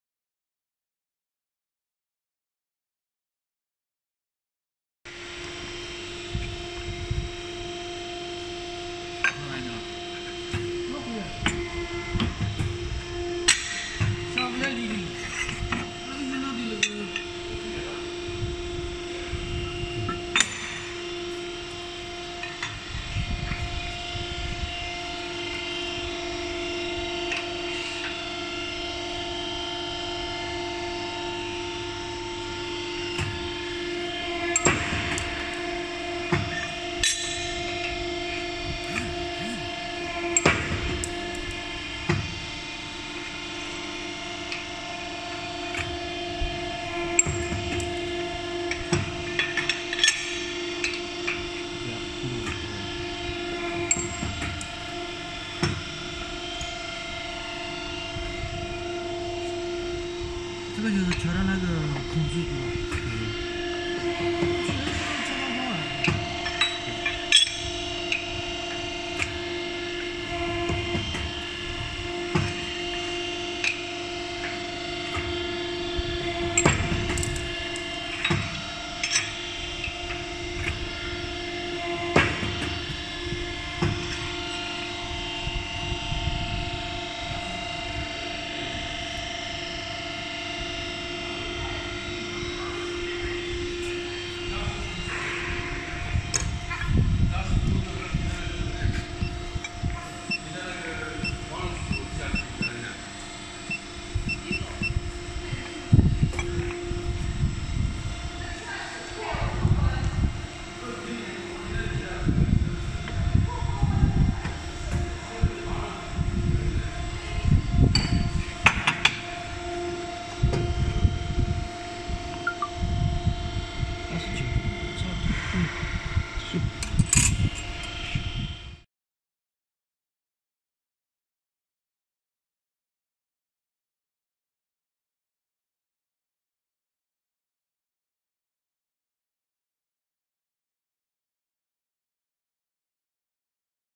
Оборудование для изготовления металлических шин
Предлагаем вашему вниманию видео работы оборудования для изготовления шин для трансформаторов и электрических подстанции. С полным описанием и характеристиками вы можете ознакомиться, посетив наш сайт